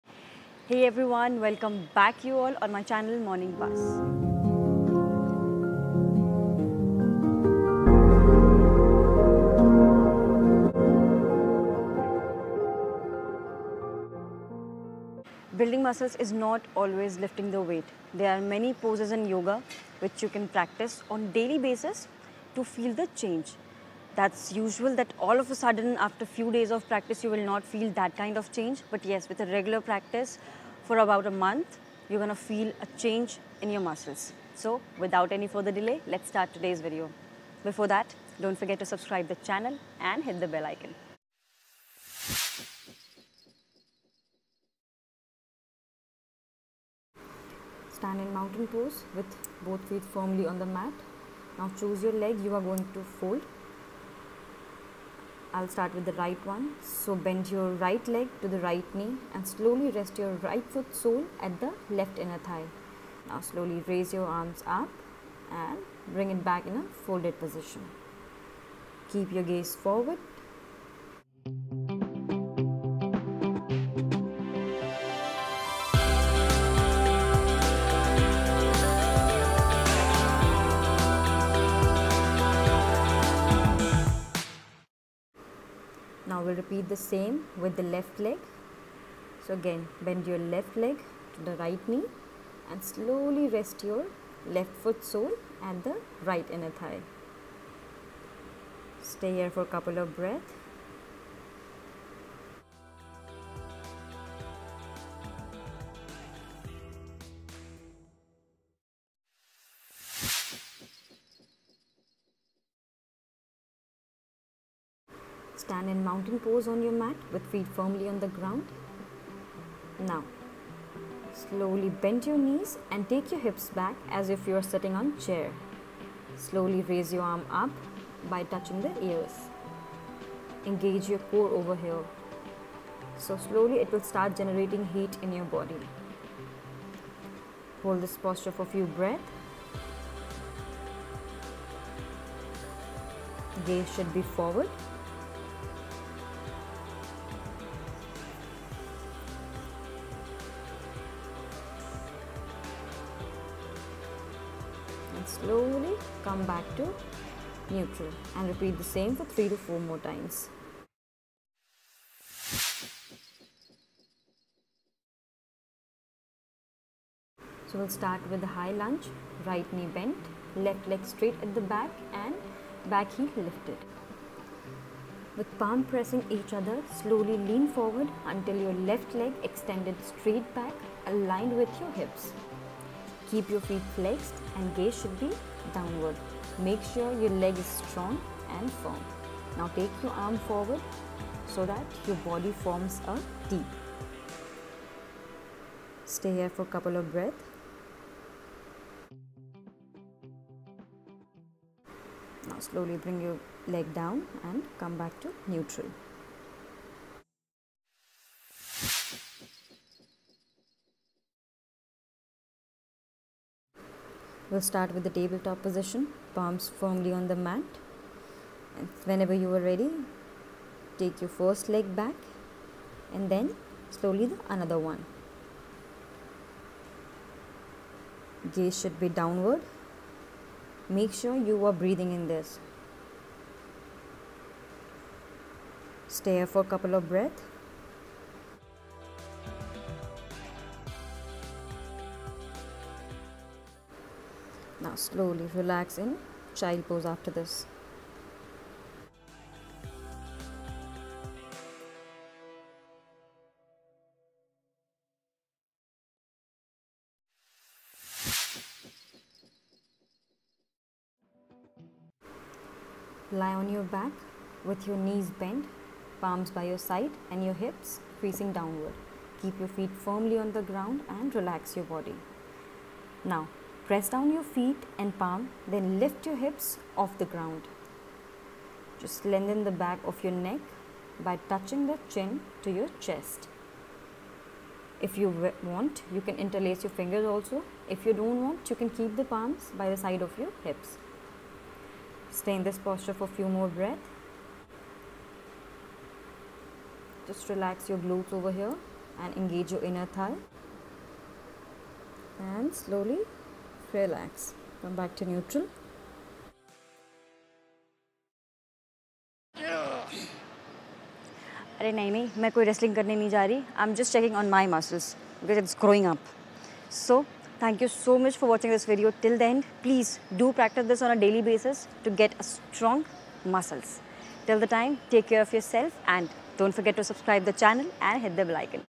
Yoga to build Muscles Strength || strong upper body muscles|| everyday yoga || Morning buzz
Building muscles is almost everyone’s dream but  people are afraid if lifting weights… we have solution here which is yoga

Include this in your daily practice  to build your muscles